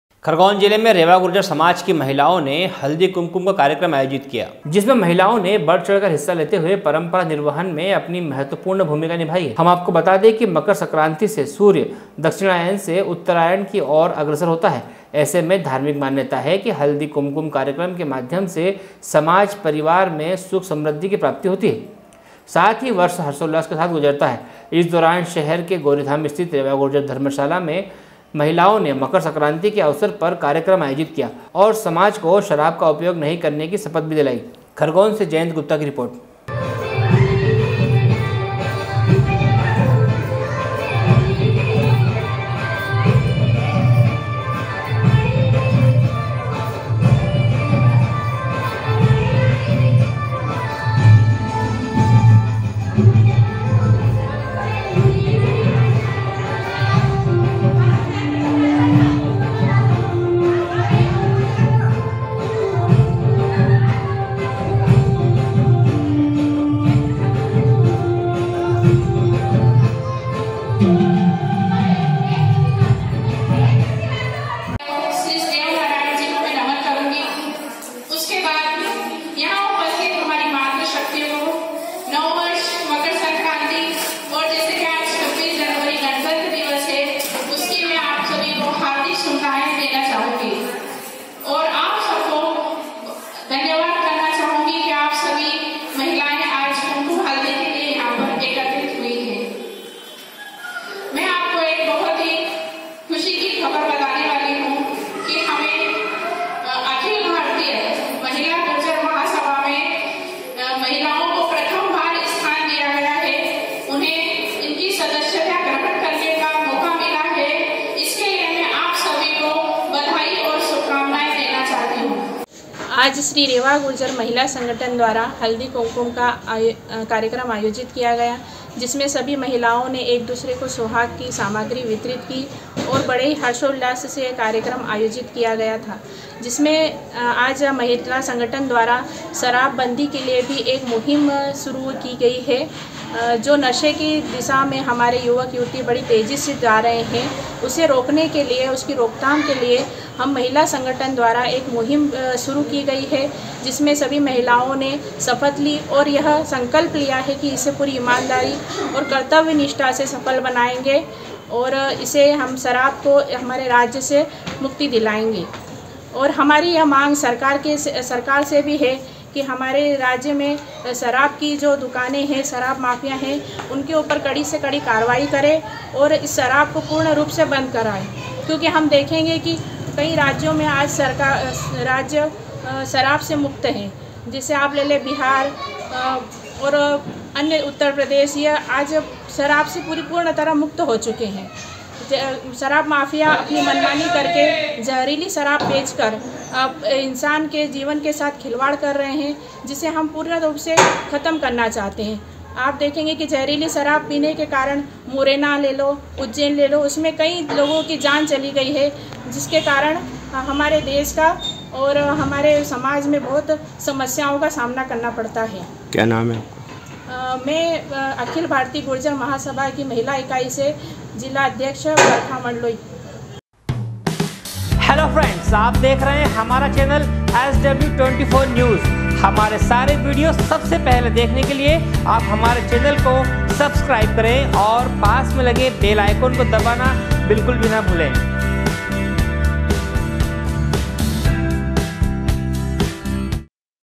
Khargone- खरगोन जिले में रेवा गुर्जर समाज की महिलाओ ने हल्दी कुमकुम का कार्यक्रम आयोजित किया
खरगोन जिले में रेवा गुर्जर समाज की महिलाओ ने हल्दी कुमकुम का कार्यक्रम आयोजित किया खरगोन से जयंत गुप्ता की रिपोर्ट